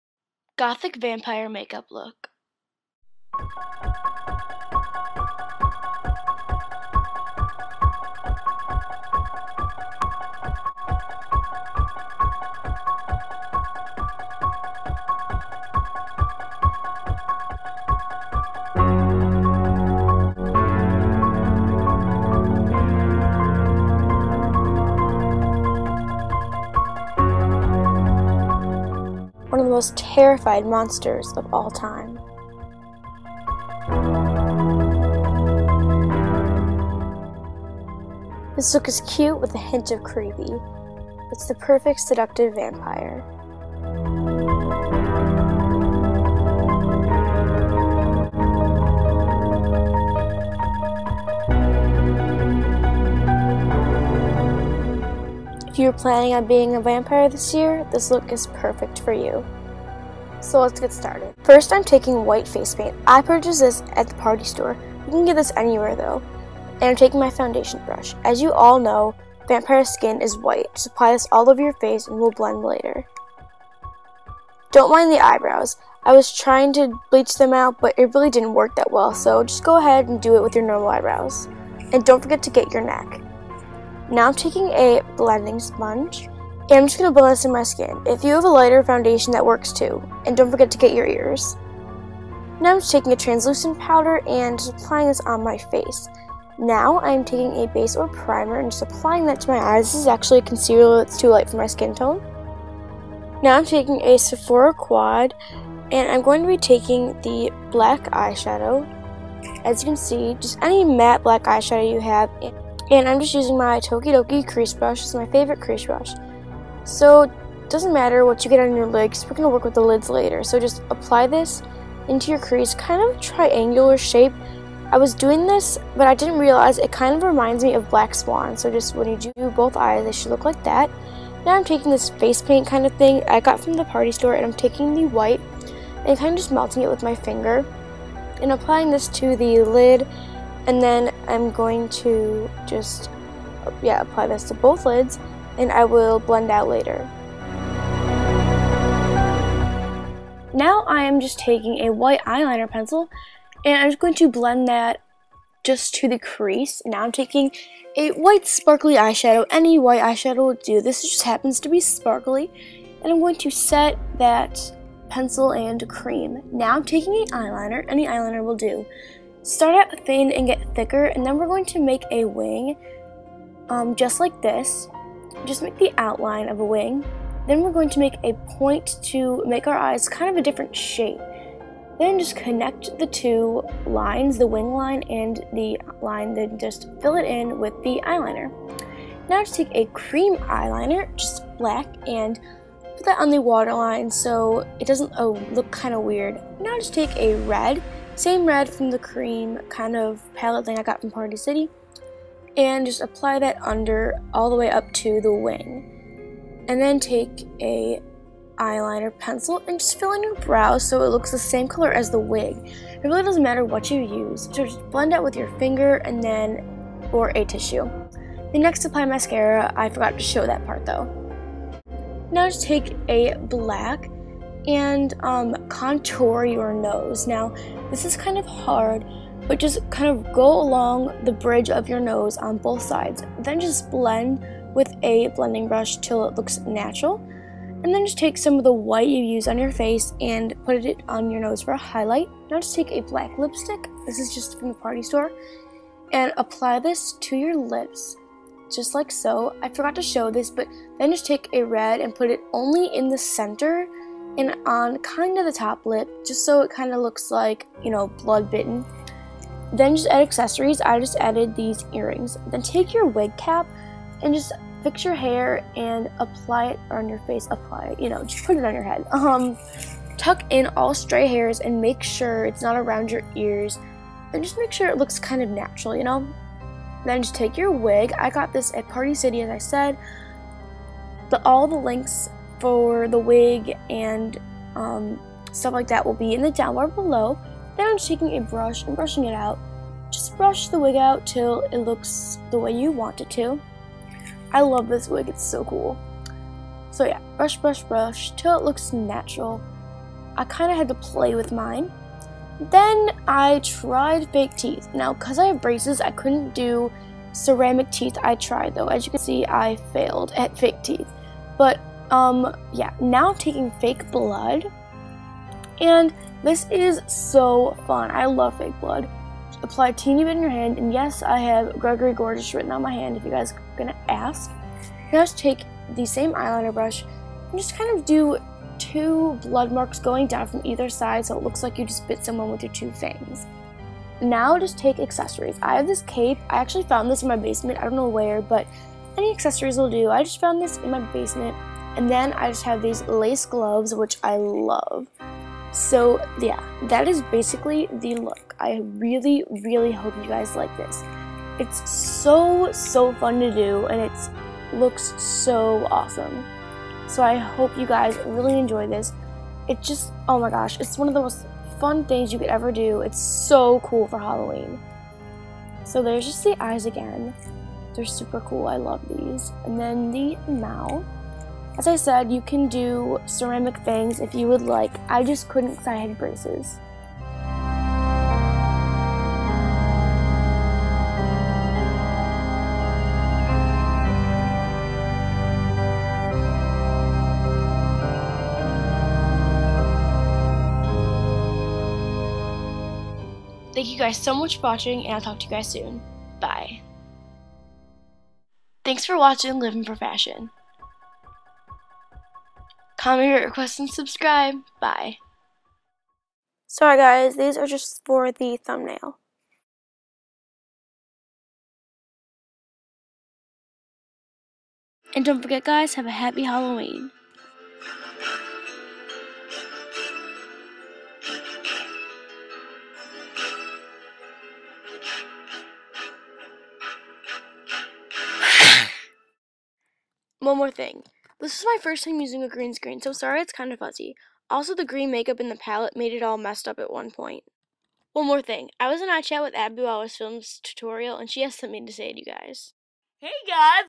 Gothic Vampire Makeup Tutorial
this is just my take on a gothic vampire :) I loved xBeautyyyx Vampire look and HAD to do one to. i really hope you guys like this look, im sorry the green screens kinda fuzzy but its cause the shaddow and green makeup. but this took me 2 hours to film so...:P yupp! This look is perfect for Halloween!